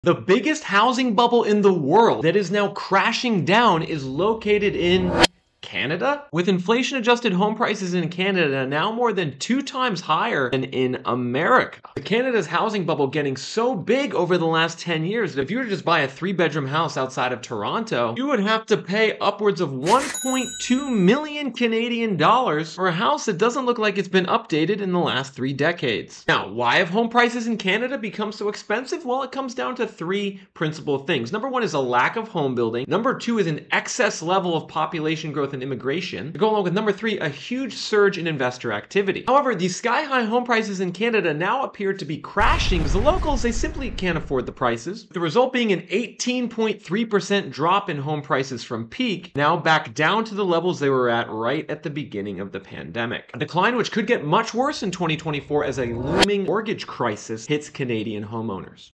Biggest Housing Bubble in the world is in....Canada?
The Biggest Housing Bubble in the world is in Canada. Where home prices adjusted for income and inflation have surged to record levels over the last several years.

Home prices in Canada are more than 2x higher than America in inflation-adjusted terms. However, they have started to come down over the last two years, with prices now down -18% from their recent peak.

However, prices in Canada are still very high. With homes outside of Toronto routinely costing more than $1 million Canadian Dollars. Despite not being renovated in decades.